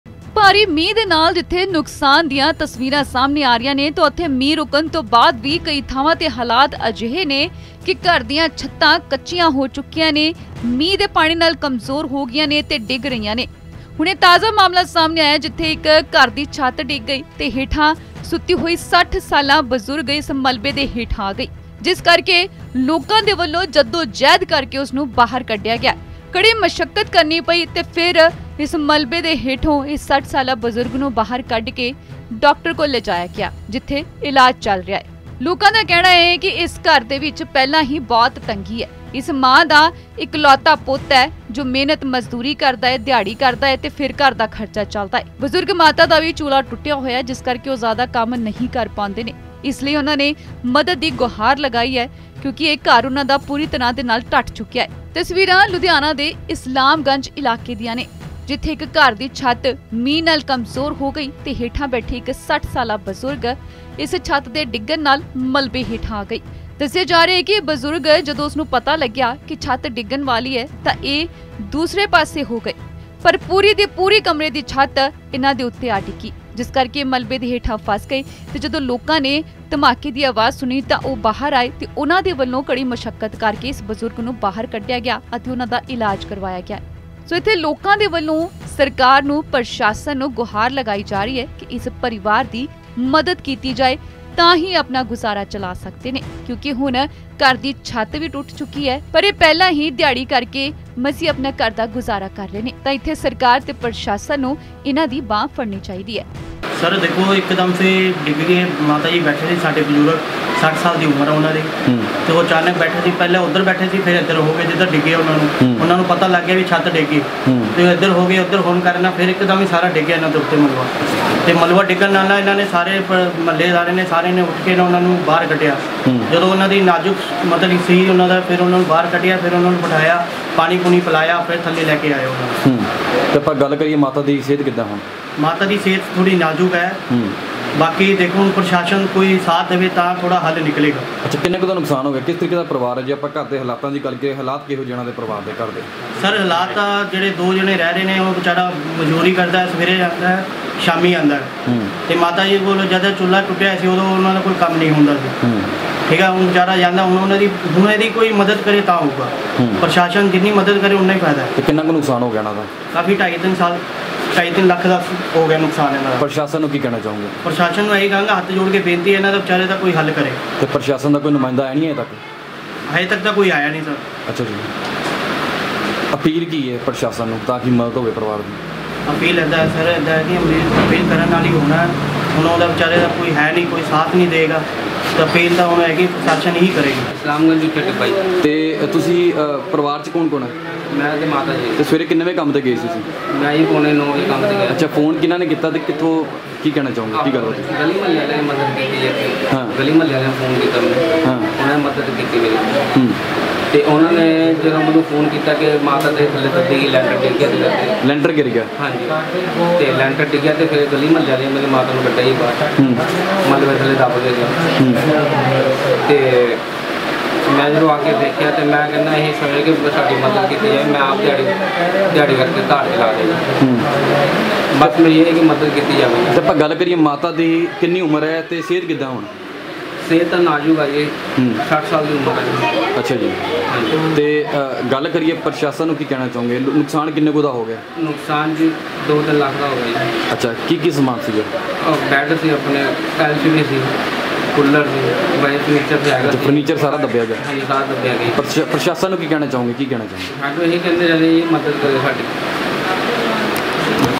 Ludhiana News | ਘਰ ਦੀ ਛੱਤ ਡਿੱਗਣ ਨਾਲ ਮਲਬੇ ਹੇਠਾਂ ਦੱਬੀ 60 ਸਾਲਾਂ ਬਜ਼ੁਰਗ, ਮੱਚ ਗਿਆ ਚੀਕ-ਚਿਹਾੜਾ
ਲੁਧਿਆਣਾ ਦੇ ਇਸਲਾਮਗੰਜ ਇਲਾਕੇ 'ਚ ਘਰ ਦੀ ਛੱਤ ਡਿੱਗਣ ਦੇ ਕਾਰਨ 60 ਸਾਲ ਦੀ ਬਜ਼ੁਰਗ ਮਹਿਲਾ ਮਲਵੇ ਹੇਠਾਂ ਦੱਬ ਗਈ, ਜਿਸ ਨੂੰ ਕੜੀ ਮਸ਼ੱਕਤ ਤੋਂ ਬਾਅਦ ਬਾਹਰ ਕੱਢਿਆ ਗਿਆ ਅਤੇ ਉਸ ਦੇ ਇਲਾਜ ਕਰਵਾਇਆ ਗਿਆ। ਦੱਸ ਦਈਏ ਕਿ ਸਵੇਰ ਦੇ ਸਮੇਂ ਇਹ ਘਟਨਾ ਹੋਈ ਹੈ।